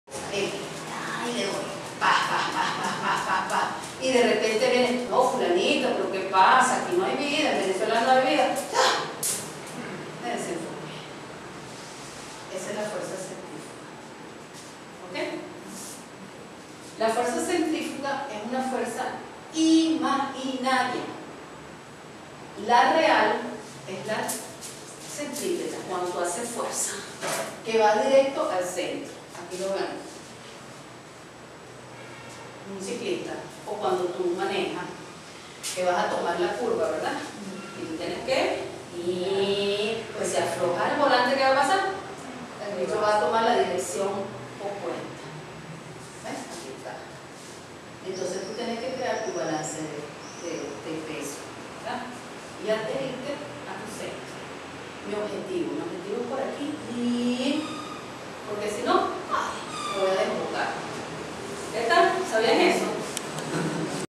Nuestras Fuerzas Circulares
Bases fundamentales de los Talleres de Inspiración del Ser
Lis Majetic
Aprende a vestir sin tocar con las velocidades del ser en tu esfera de vida